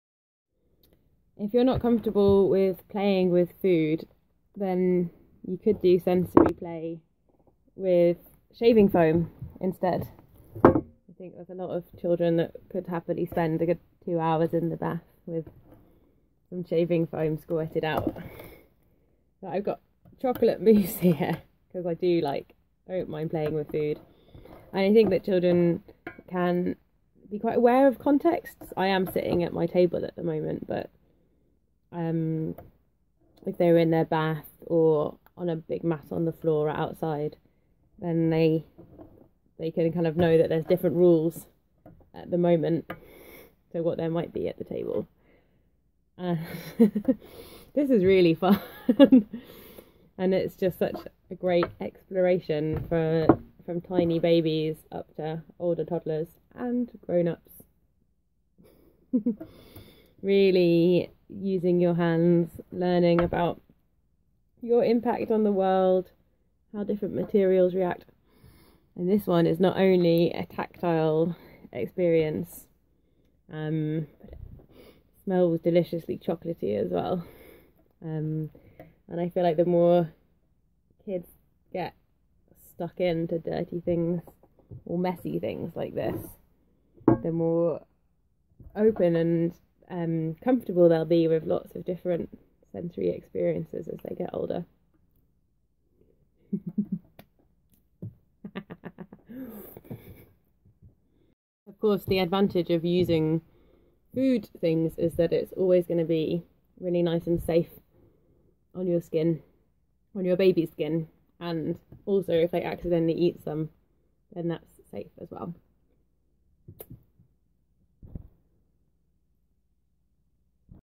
Sensory Play for Babies
You might be comfortable playing with food, and there is a solution: use shaving foam, if you think that works best with your baby! This is really fun! Do try it at home!